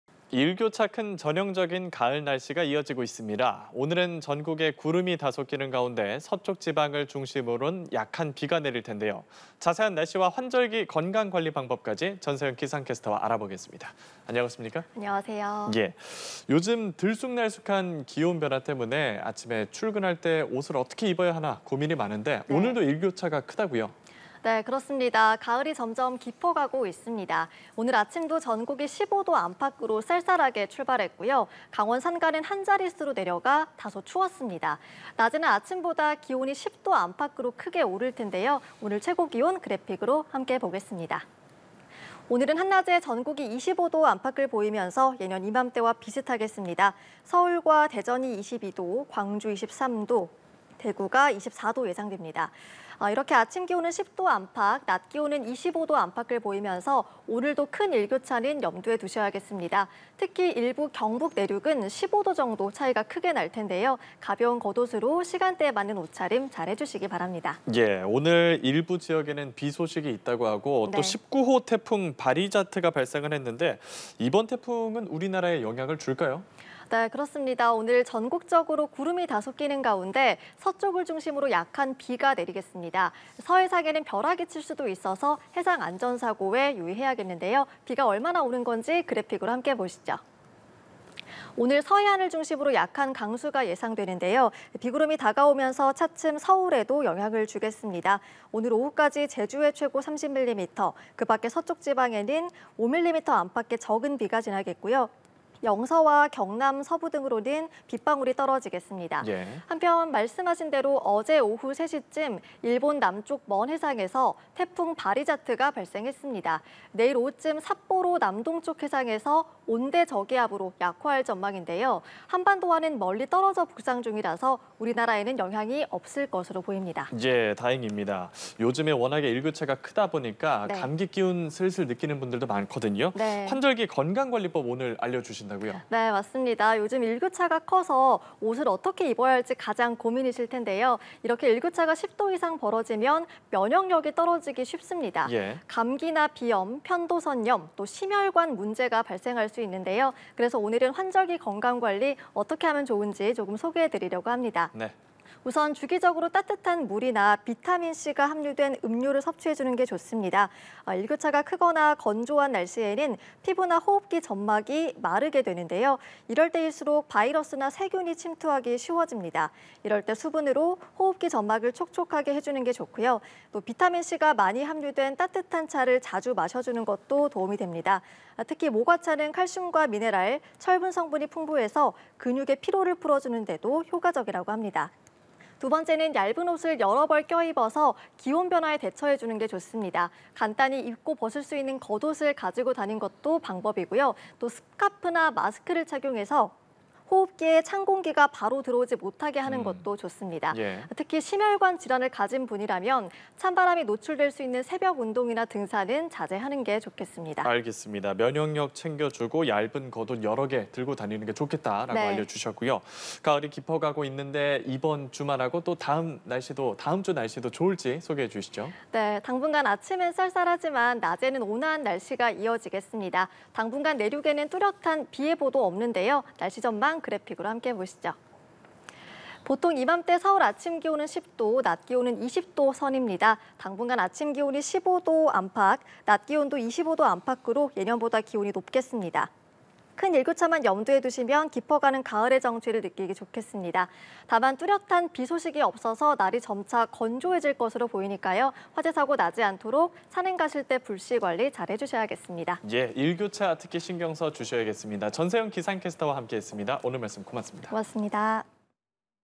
[센터웨더] 전국 구름 많고 서쪽지방 비…환절기 건강관리 주의 / 연합뉴스TV (YonhapnewsTV)
[센터웨더] 전국 구름 많고 서쪽지방 비…환절기 건강관리 주의
[출연 : 전세영 기상캐스터]

일교차 큰 전형적인 가을 날씨가 이어지고 있습니다.

오늘은 전국에 구름이 다소 끼는 가운데 서쪽 지방을 중심으로는 약한 비가 내릴 텐데요.

자세한 날씨와 환절기 건강관리 방법까지, 전세영 기상캐스터와 알아보겠습니다.

[질문 1] 요즘 들쑥날쑥한 기온 변화 때문에 옷차림 고민도 커지고 있는데요. 오늘도 아침에는 쌀쌀했는데, 낮에는 기온이 올라 일교차가 크게 난다고요?

[질문 2] 오늘 일부 지역에 비 소식이 있다고요? 또 19호 태풍 '바리자트'가 발생했는데 우리나라에는 영향이 없는 거죠?

[질문 3] 요즘 큰 일교차 탓에 감기에 걸린 분들도 많은데요. 환절기 건강 관리, 어떻게 하면 좋을까요?

[질문 4] 가을이 완연해 가고 있습니다. 이번 주말, 또 다음 주 날씨 전망도 전해주시죠.